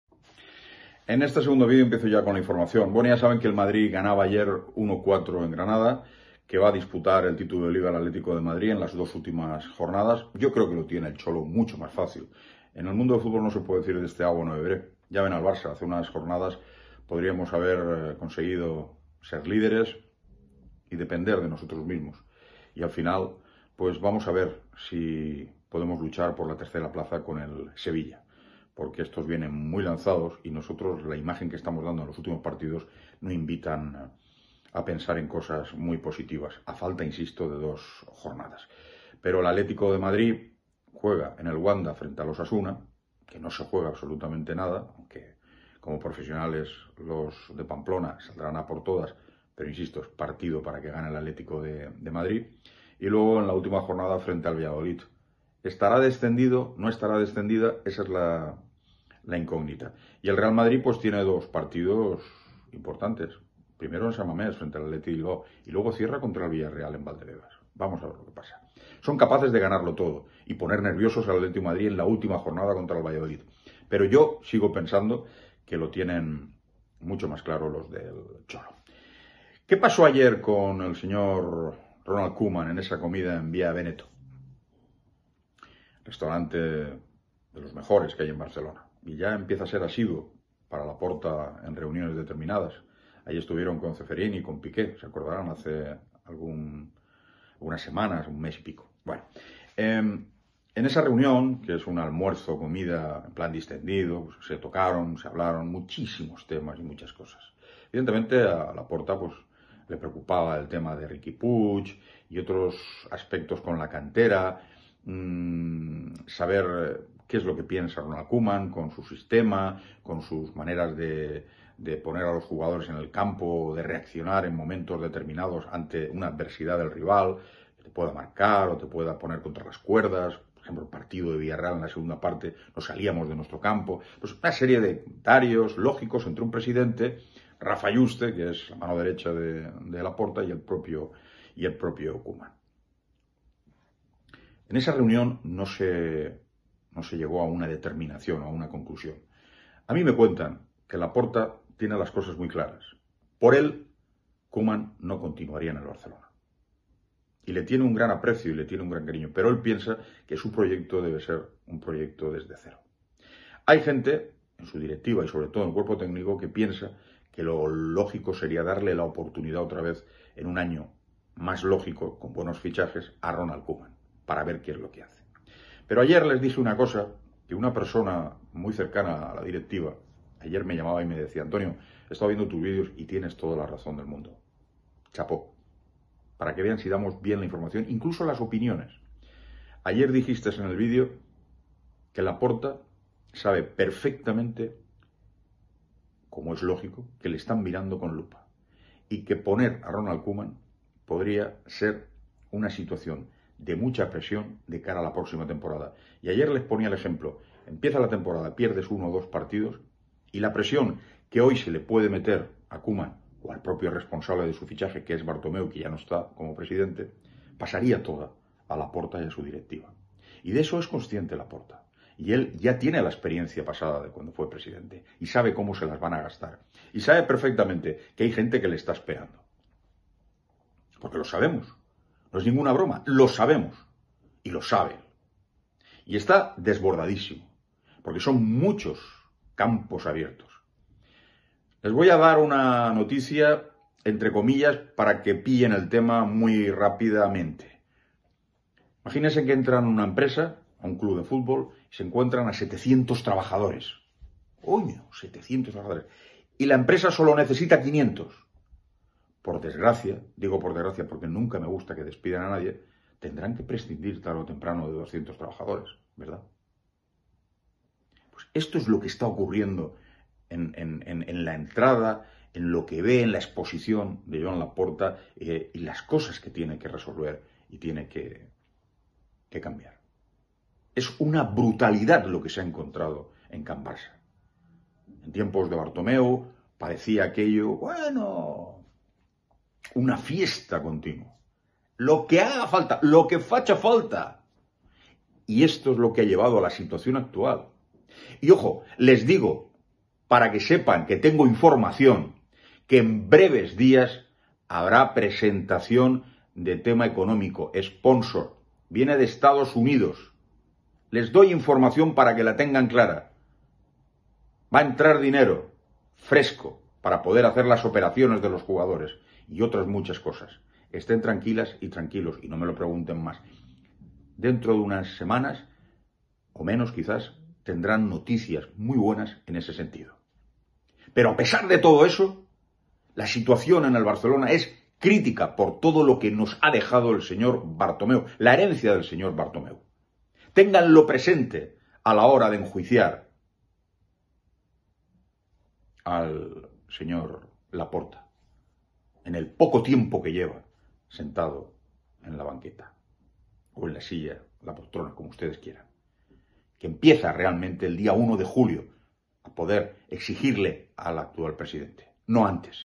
☀ LAPORTA está desbordado ☀ || ¿ Quién será el ENTRENADOR para el próximo BARÇA ????
LAPORTA  está desbordado
LAPORTA  tiene mucha presión a la hora de decidir qué técnico estará la próxima temporada!
De todas maneras, tiene tomada la decisión👌👌

Información / Opinión sobre el FCBarcelona​​​